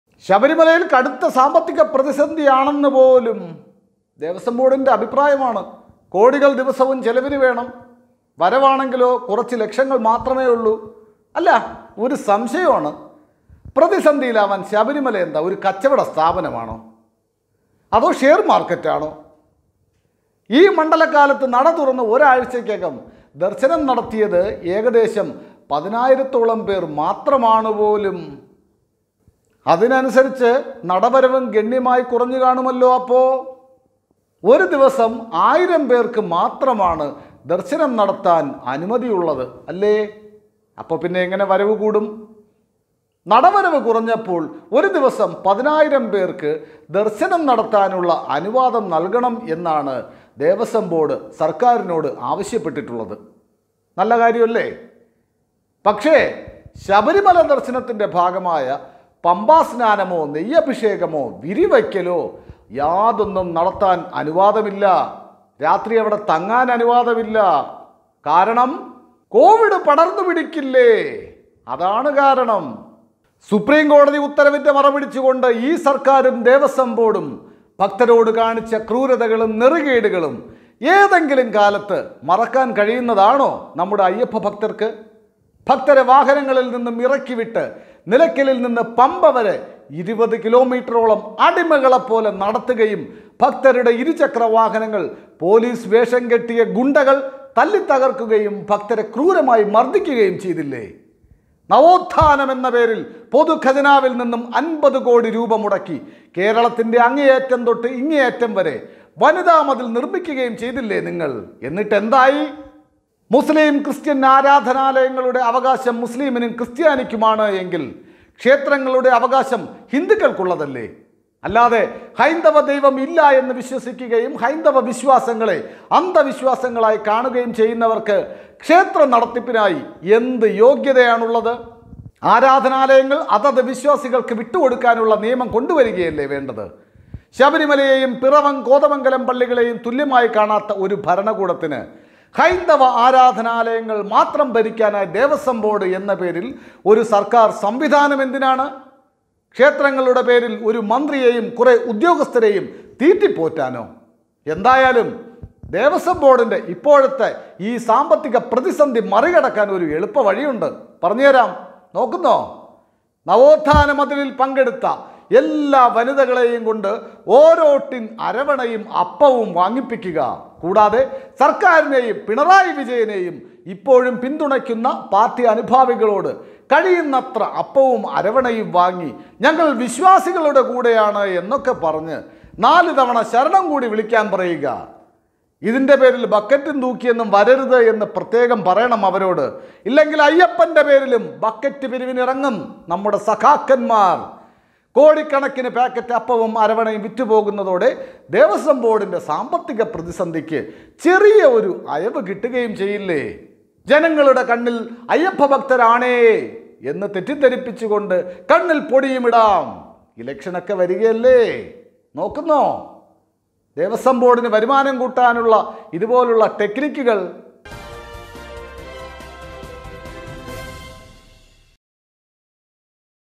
ശബരിമലയിൽ വരുമാന നഷ്ടമെന്ന് കടകംപള്ളി..ഇത് ക്ഷേത്രമെന്ന് തന്ത്രി
കടകംപള്ളിയിലെ ആ വികടസരസ്വതി മൊഴിഞ്ഞപ്പോൾ ഉള്ളിലിരുപ്പ് ദാ...ലോകമറിഞ്ഞു